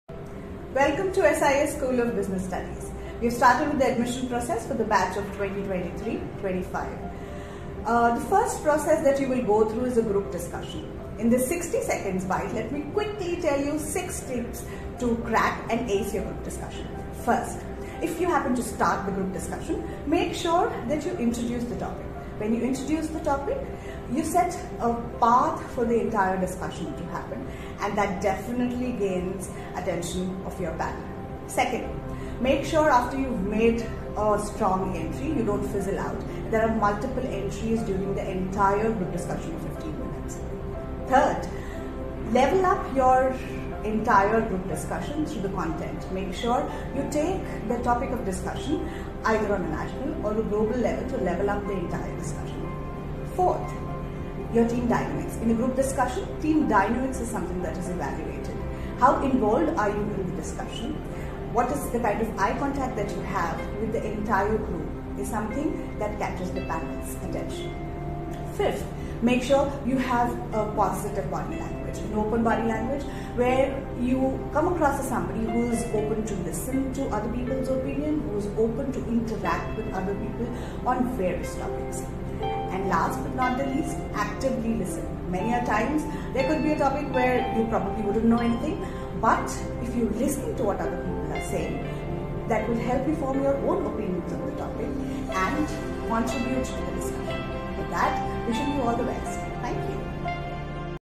Tips to ace the Group Discussion || SIESSBS || ADMISSIONS OPEN 2023
Feeling stuck in a group discussion rut? Here are a few tips by Prof. Sanchi Bhide, Manager - Placements, to ace the group discussions.

Admissions Open for the PGDM Batch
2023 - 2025